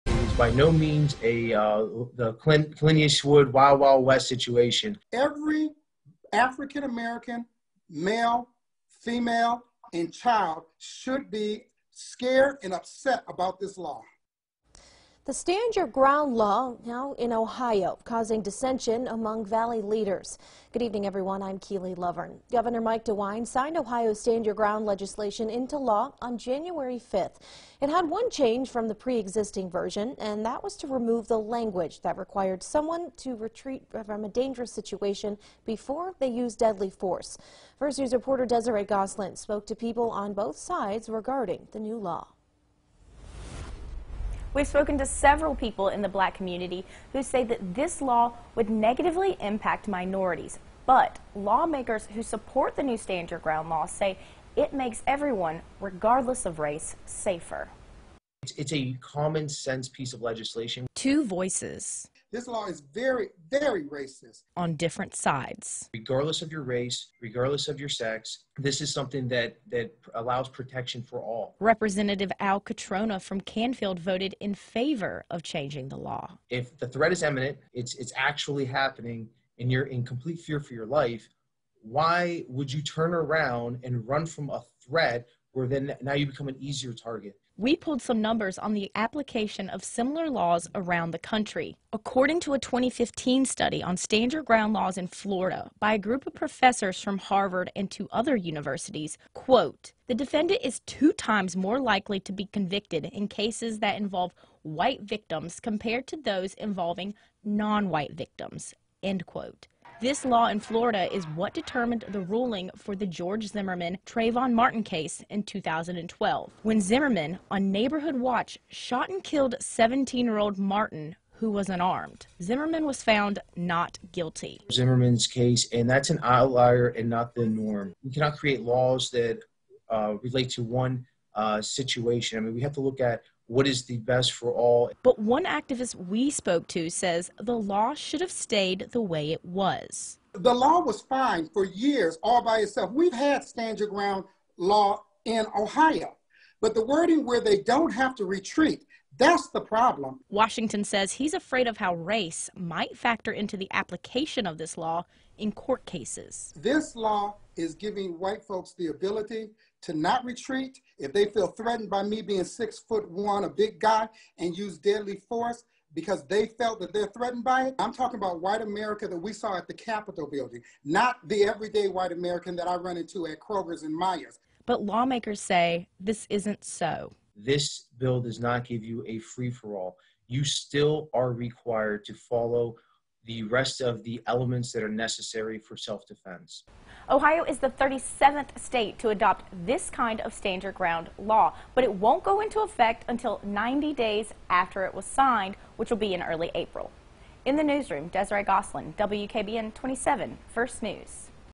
Lawmakers, activists on opposites sides over ‘Stand Your Ground’ law
Ohio is the 37th state to adopt this kind of Stand Your Ground law